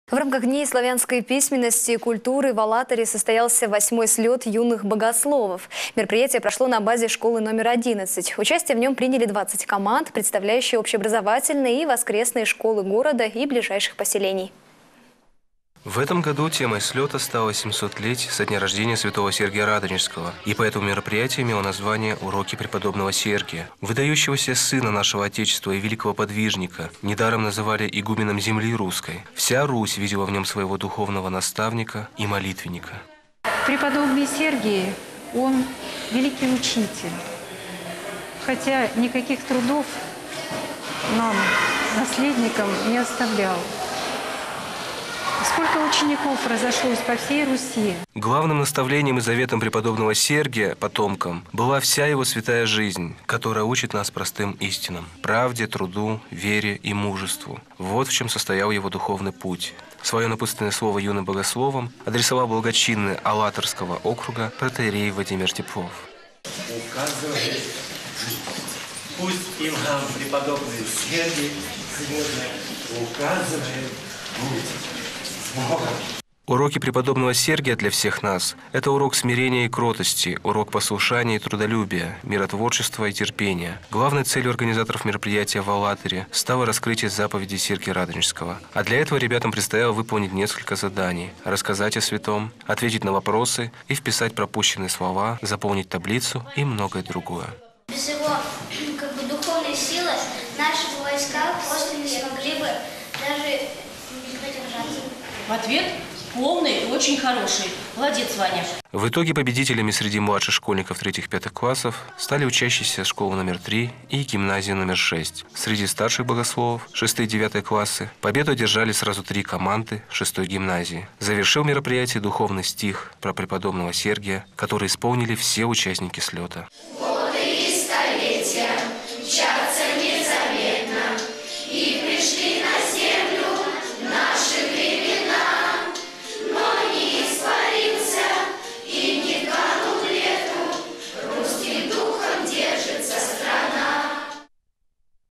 VIII слёт юных богословов в Алатыре
В рамках Дней славянской письменности и культуры в Алатыре состоялся VIII слёт юных богословов. Мероприятие прошло на базе школы №11. Участие в нём приняли 20 команд, представлявшие общеобразовательные и воскресные школы города и ближайших поселений.